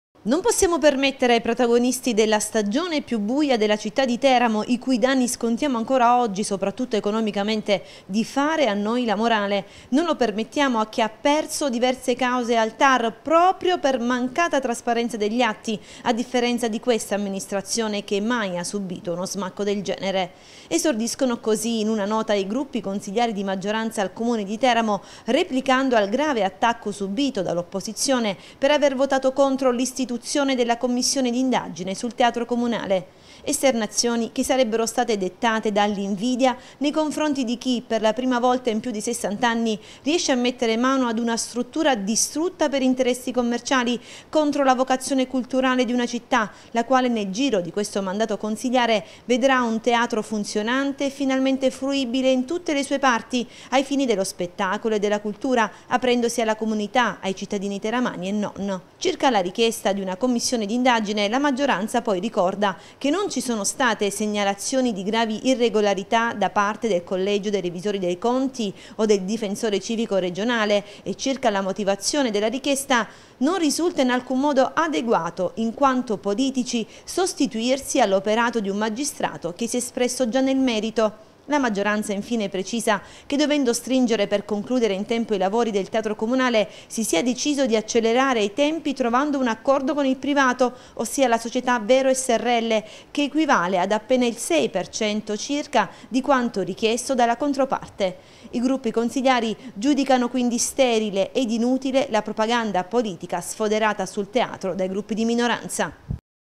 Teramo | Teatro comunale, replica la maggioranza: "Polemica sterile e fuorviante"
| SUPER J |

Contatti Redazione:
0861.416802